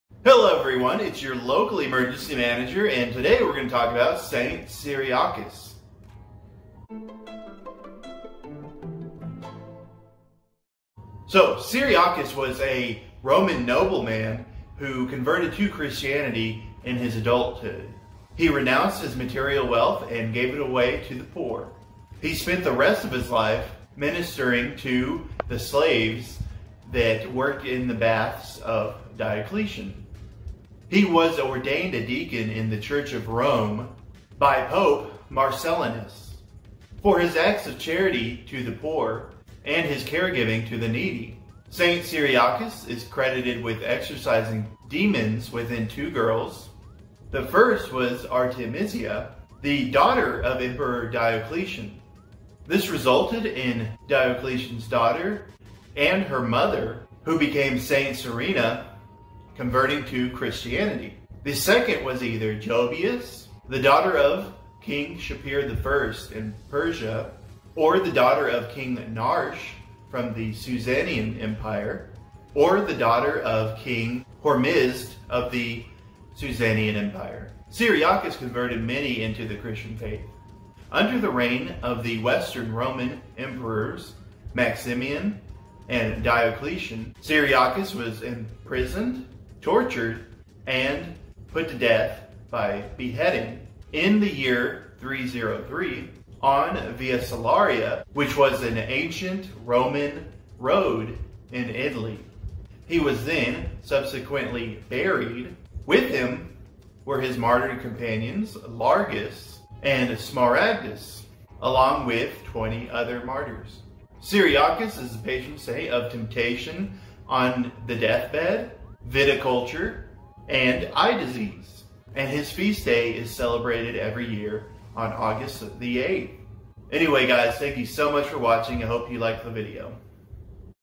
August 8: St. Cyriacus
August 8th is St. Cyriacus' feast day. Check out this video to learn all about him.